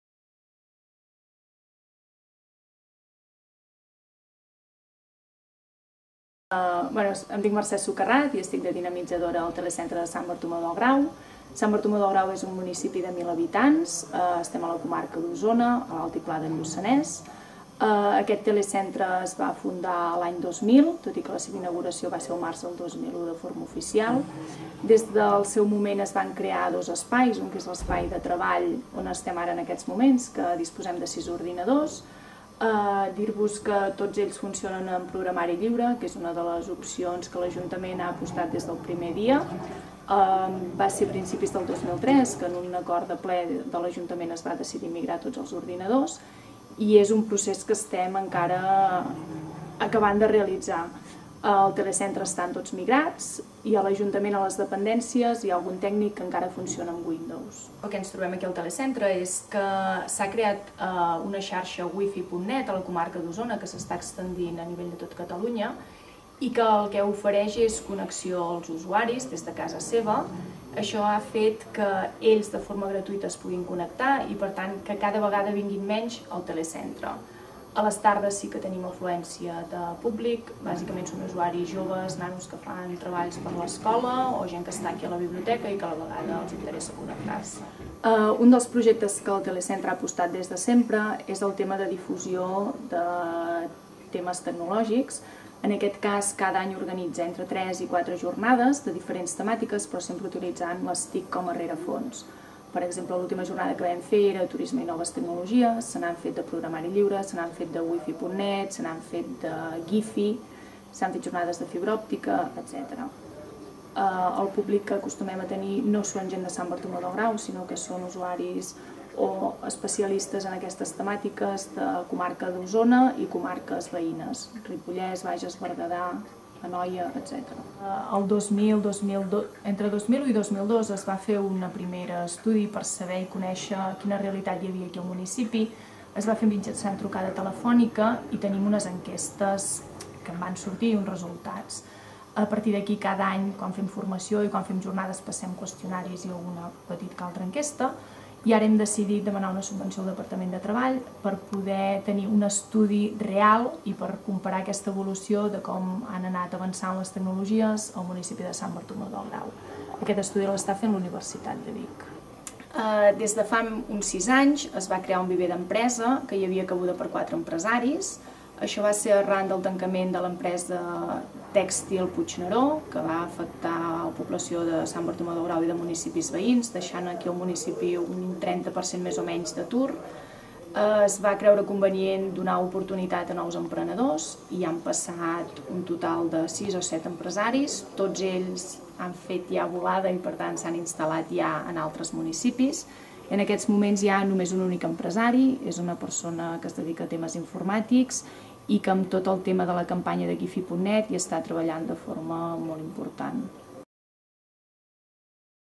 Mercè Sucarrats ens apropa al Punt TIC de Sant Bartomeu del Grau
El Punt TIC de Sant Bartomeu del Grau ha estat present i ha sigut actor important en diversos esdeveniments que han afectat el municipi, sempre impulsant la Societat de la Informació i intentant buscar les solucions TIC més adients. Mercè Sucarrats, dinamitzadora del punt, ens explica el seu passat, present i futur.
La Mercè és la dinamitzadora del Punt TIC de Sant Bartomeu del Grau des de fa uns anys. Hi treballa amb total implicació, sempre buscant formes d'innovar i de donar nous usos a les TIC que siguin útils per als ciutadans i ciutadanes de Sant Bartomeu del Grau i de la resta de la comarca. En aquest vídeo, ens parla de la trajectòria del punt, de les diverses iniciatives que porten a terme i dels projectes de futur.